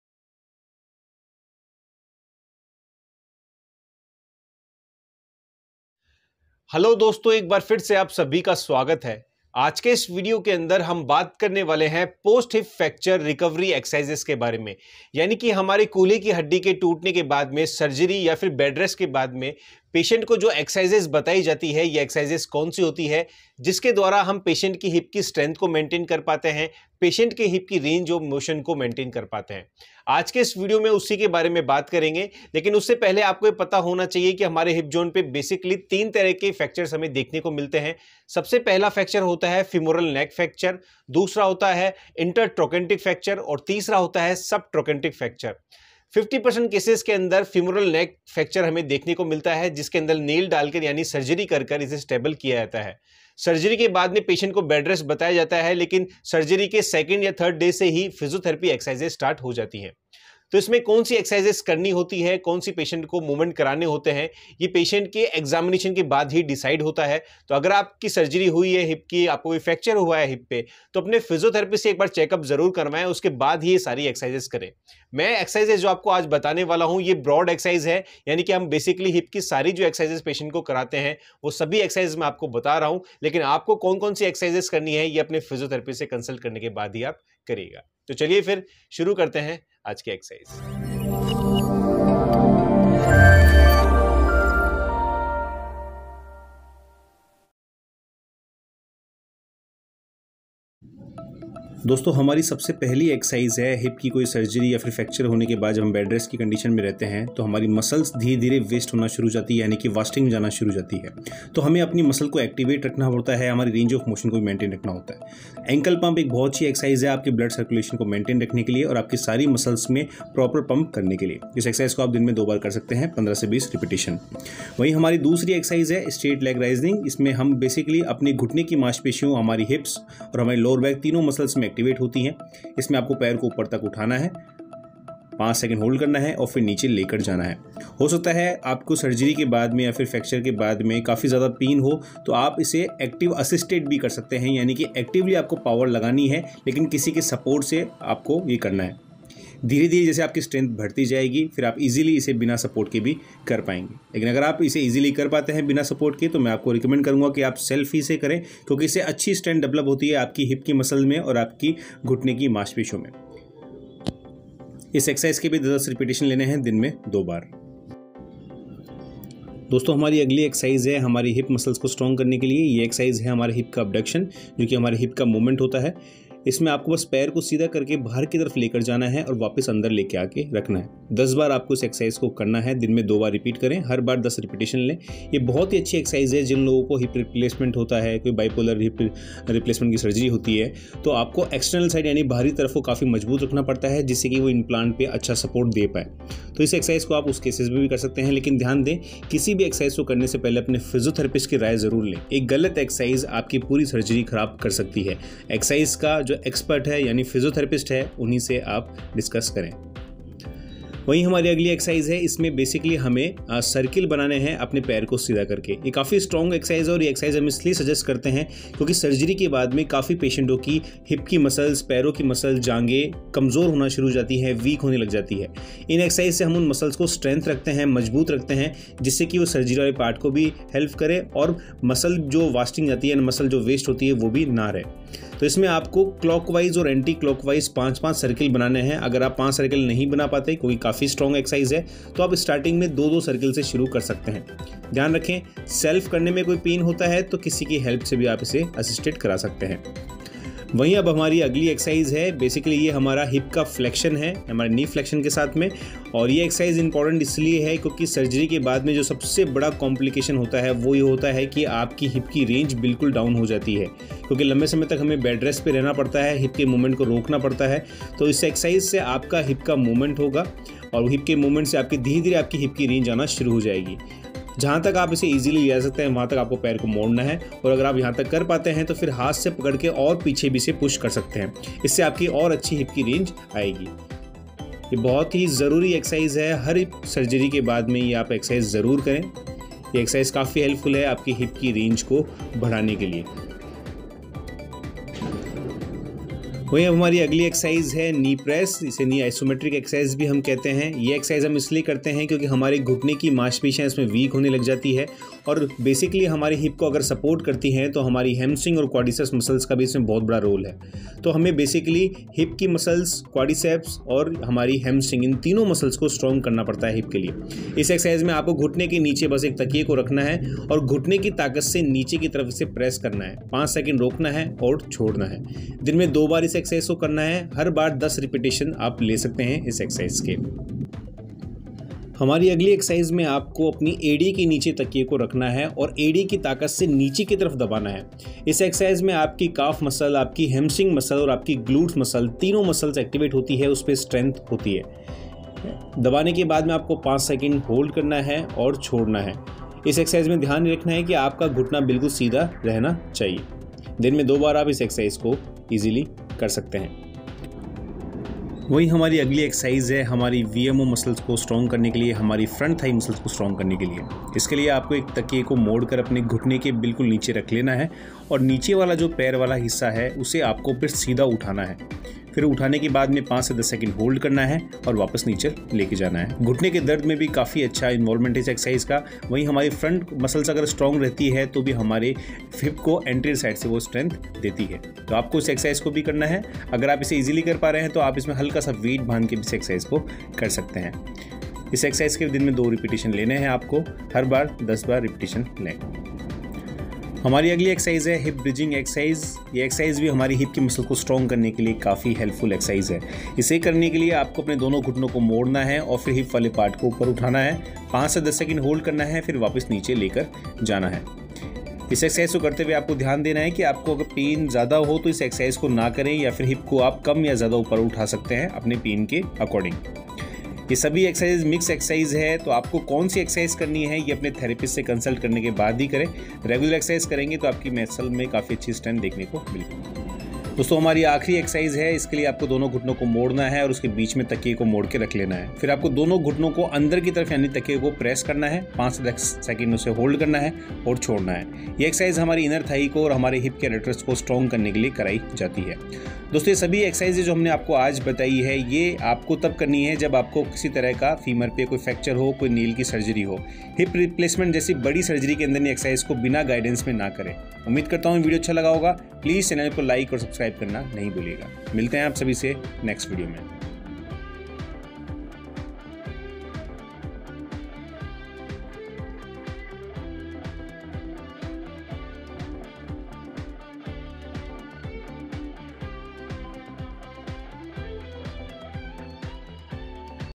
Post hip fracture recovery exercises | कूल्हे की हड्डी टूटने के बाद की एक्सरसाइज | Dr Sunil Tank
Post hip fracture recovery Physiotherapy exercises should start immediately after the fracture has been immobilised. Physiotherapy during fracture healing will concentrate on:
Promoting healing,Encouraging weight bearing
Maintaining strength of weakened muscles
,Maintaining range of movement of the affected and surrounding joints,Reducing pain,Reducing swelling

After your fracture has healed and/or your cast has been removed physiotherapy is continued for 3-5  months or until you have regained your full level of function.

In this video Dr Sunil Tank showing some best exercises which we used in post hip fracture physiotherapy, you can do this exercises at Home.

Dr Sunil Tank
Physiopoint
G-25 RAMA HARITAGE central SPINE
Vidhyadhar nagar-Jaipur Rajasthan